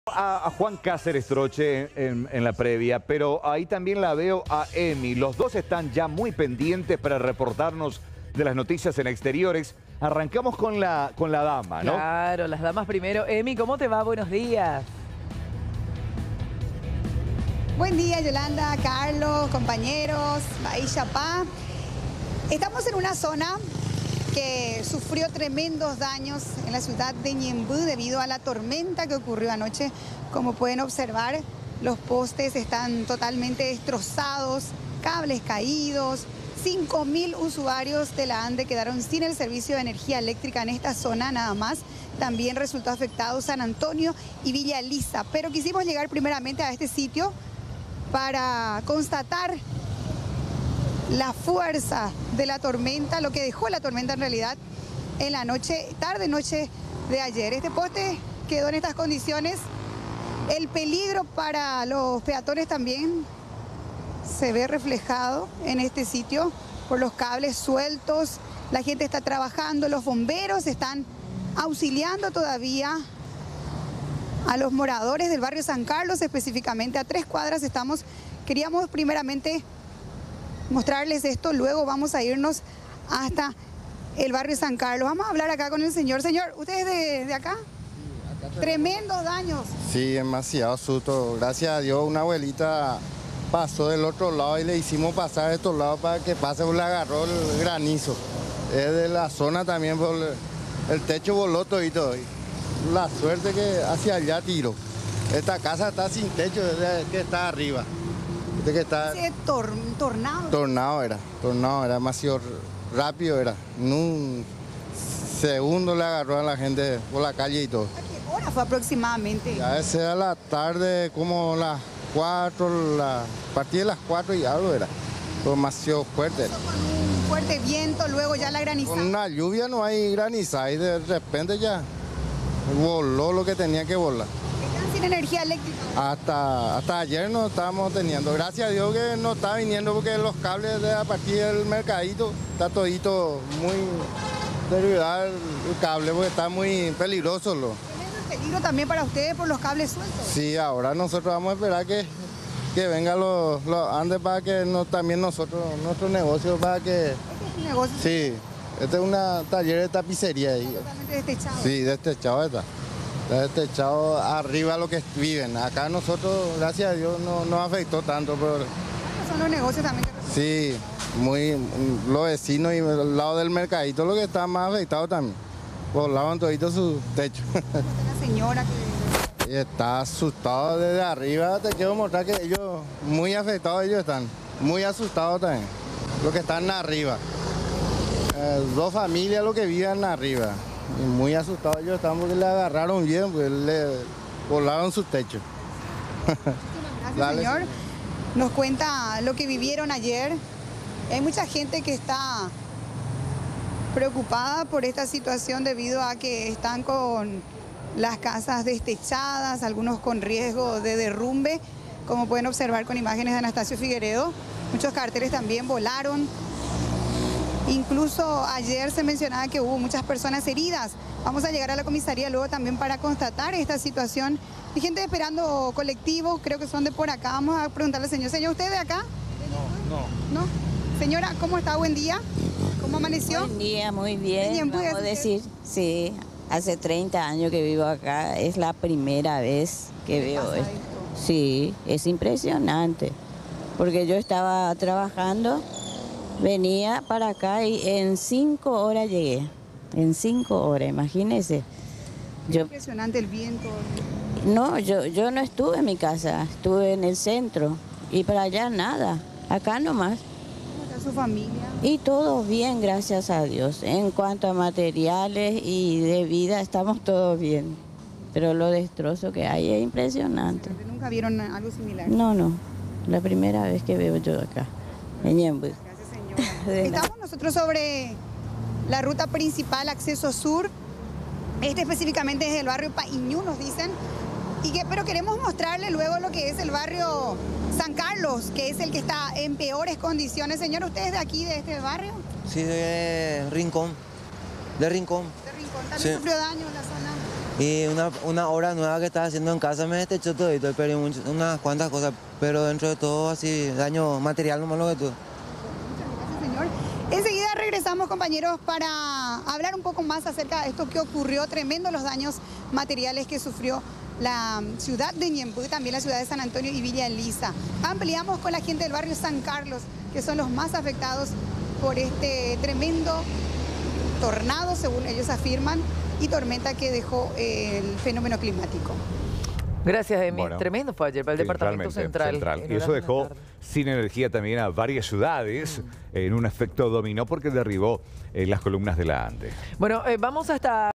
Cuantiosos daños dejó tormenta en Ñemby
Anoche se registró el fenómeno climático, que entre otras cosas dejó a más de cinco mil usuarios de la Ande sin energía eléctrica